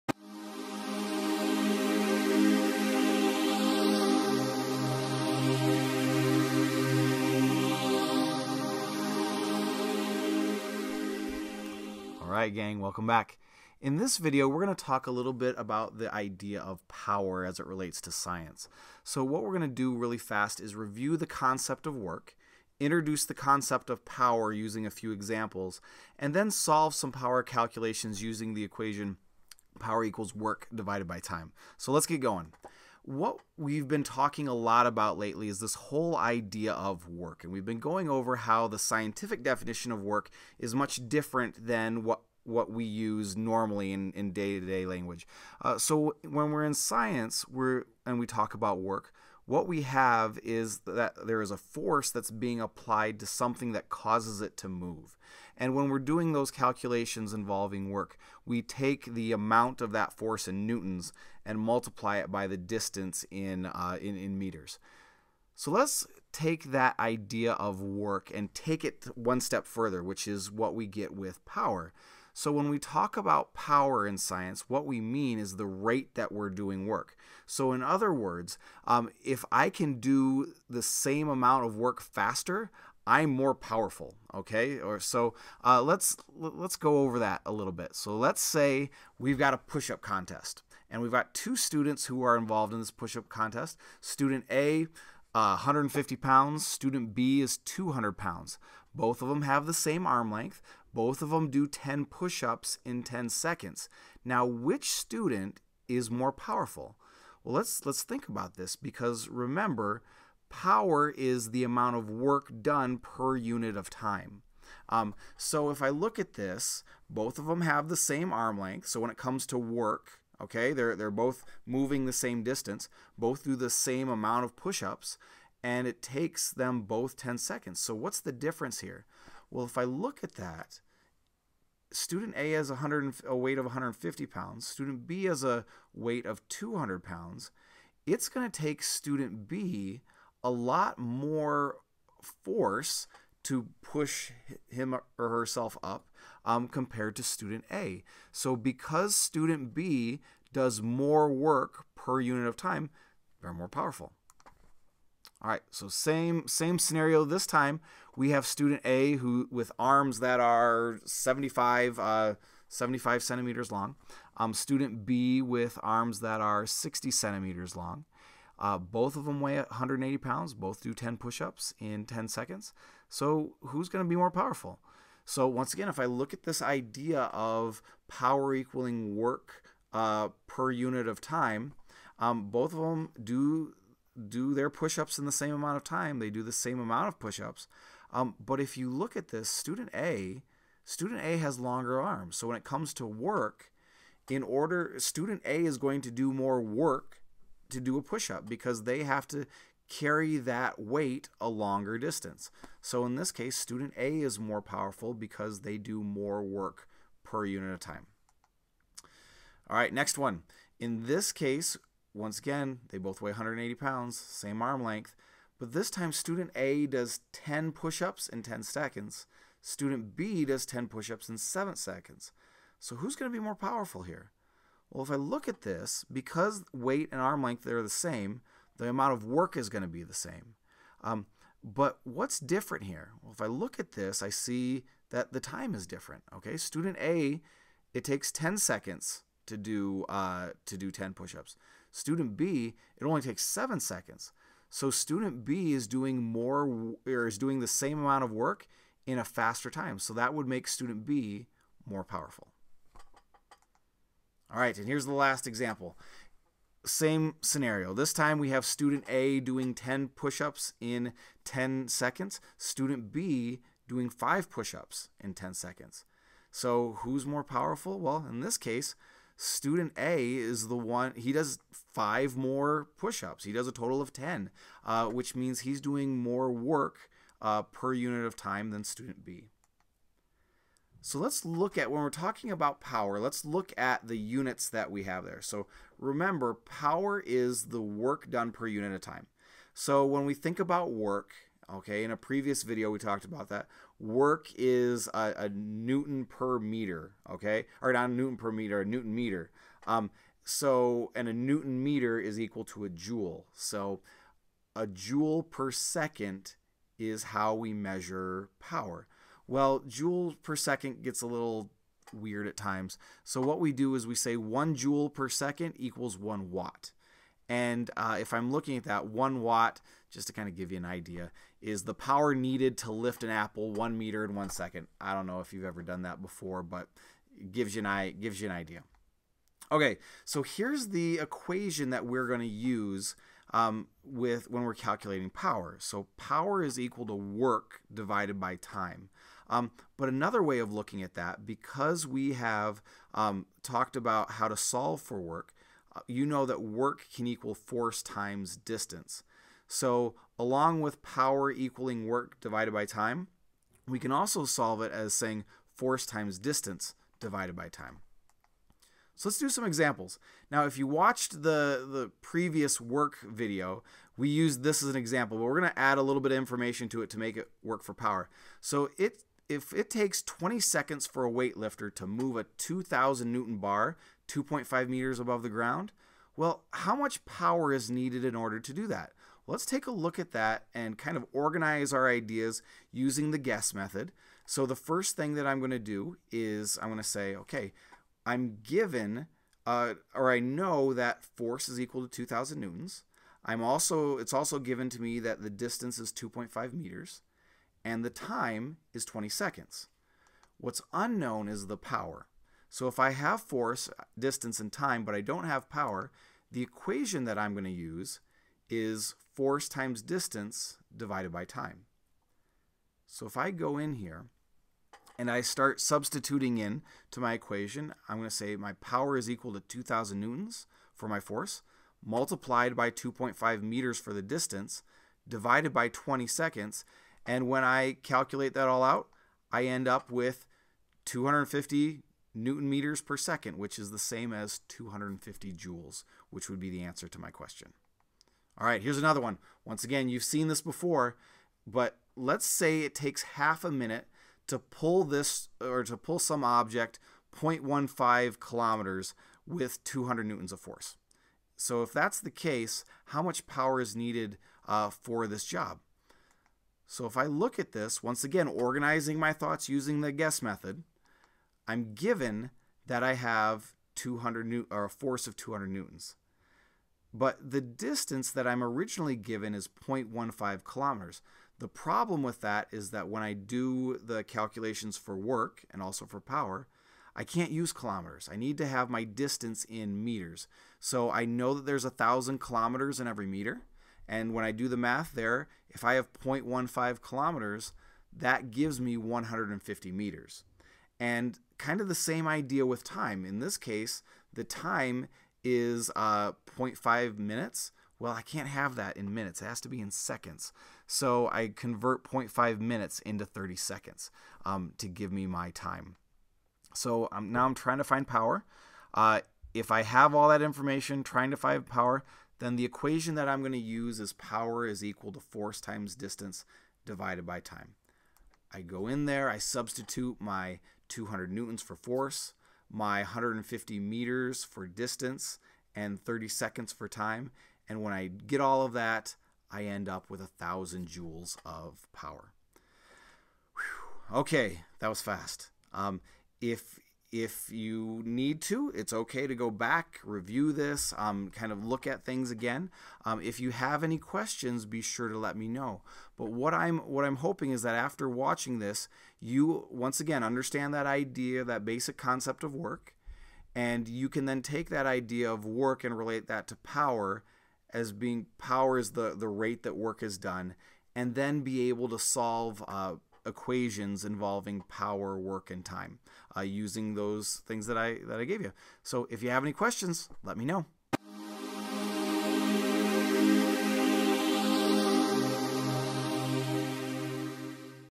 SpencerScience - Power
In this video we go over the basic concept of power and how it relates to work and time. We also work through some example power problems using the equation Power = Work / Time.

Table of Contents:
00:32 - Reviewing “Work”
01:04 - Scientific Concept of Power
04:39 - Units for Power
05:28 - Power = Work / Time -or- (Force x Distance)  Time